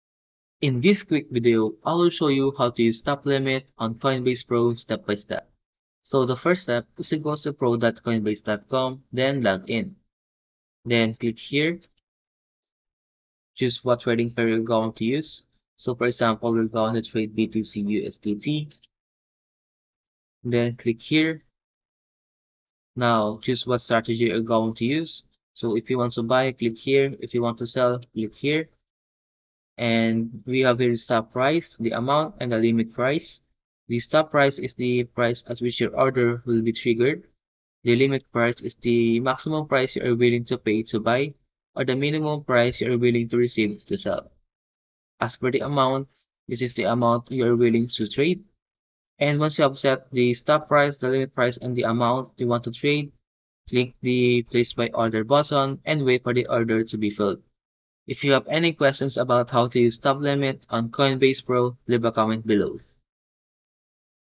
How to Use the Stop Limit on Coinbase Pro (Step by Step)
In this quick video, I will show you How to Use the Stop Limit on Coinbase Pro step by step.

"Are you new to Coinbase Pro and confused about how to use the Stop Limit feature? Don't worry, we've got you covered! In this video, we'll show you step-by-step how to use the Stop Limit on Coinbase Pro to help you manage risk and minimize losses."

"To get started, log in to your Coinbase Pro account and navigate to the trading page. From here, you can choose the Stop Limit order type from the drop-down menu."

"Next, you'll need to specify the price at which you want the Stop Limit to trigger. This will be your Stop Price. You can set this to a specific price point or use the current market price for your Stop Price."

"After setting your Stop Price, you'll need to set your Limit Price. This is the price that you want your order to fill at if your Stop Price is triggered."

"Once you have set both your Stop Price and your Limit Price, you can place your Stop Limit order. Your order will now be active until your Stop Price is reached, at which point your order will fill at your specified Limit Price."

"One thing to keep in mind when using the Stop Limit on Coinbase Pro is that you will need to be proactive in monitoring your order. If the market price moves quickly, your order may not fill at your desired Limit Price. To avoid this, you can use a wider price range for your Stop Price and Limit Price."

"And there you have it! With these simple steps, you can use the Stop Limit feature to help manage your risks and minimize losses on Coinbase Pro. Happy trading!"